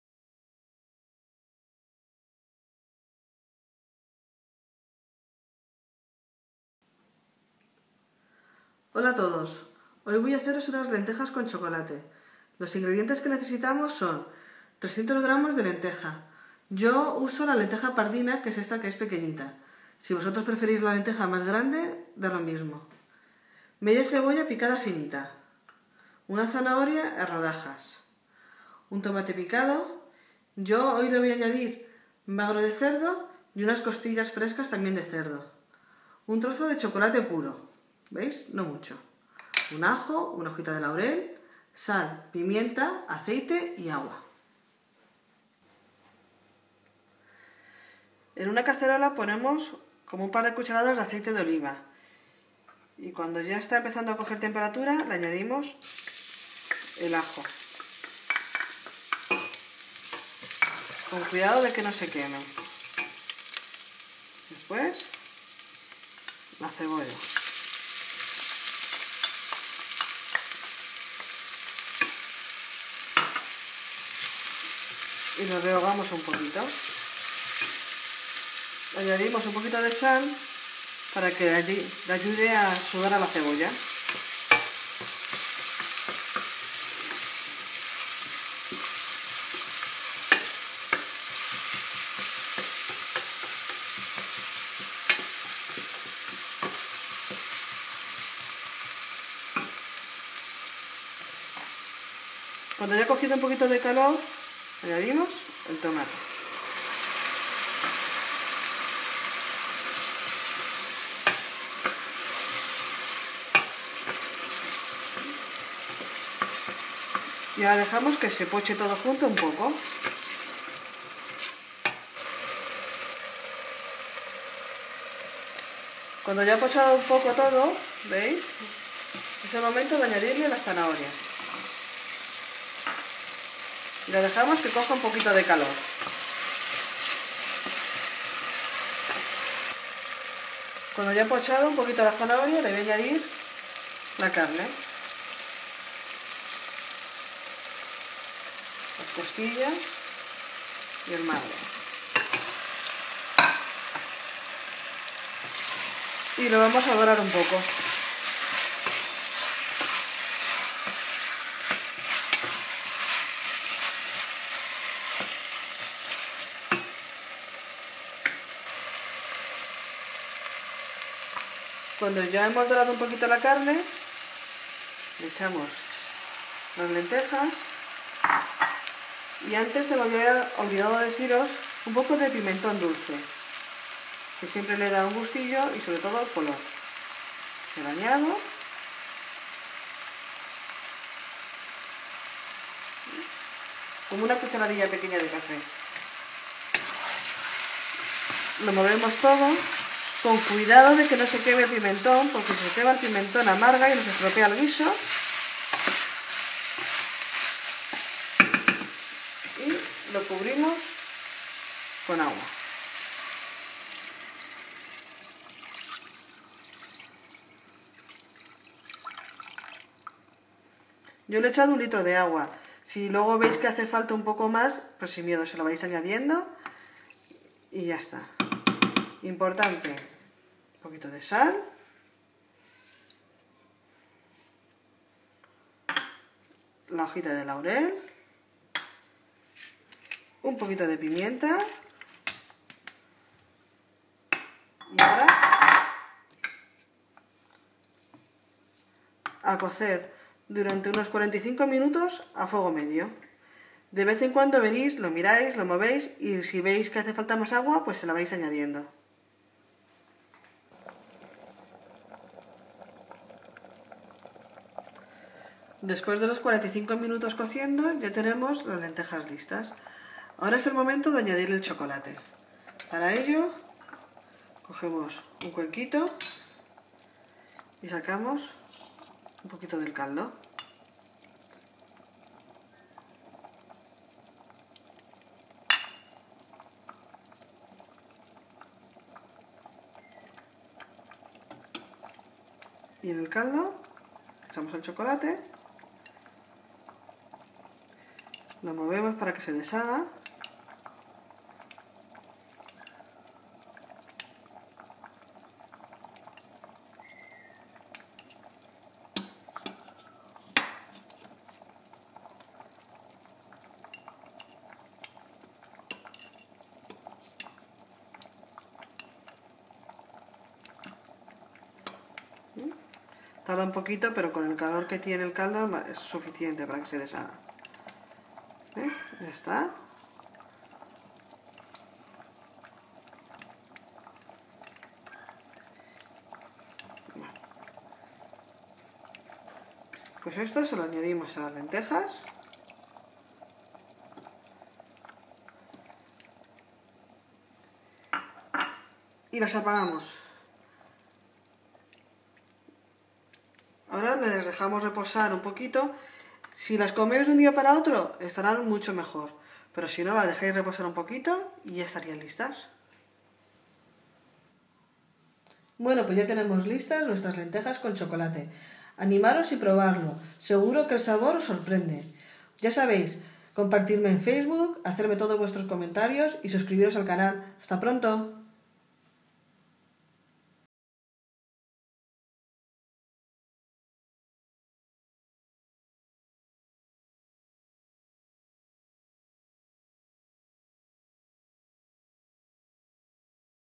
Lentejas con chocolate
Animaros a probarlo, tiene un toque distinto que os gustará.

Si te gusta el vídeo por favor dale a "ME GUSTA", y/o "COMPARTIR" así los demás también podrán disfrutar de la receta.

Ingredientes:

300 gr. de lentejas.
Media cebolla.
1 zanahoria.
1 tomate.
500 gr. de magro de cerdo o costillas de cerdo.
1 trozo de chocolate puro.
1 diente de ajo.
1 hoja de laurel
1 cucharadita de pimentón dulce.
Agua, aceite de oliva, sal y pimienta.

*Video resubido*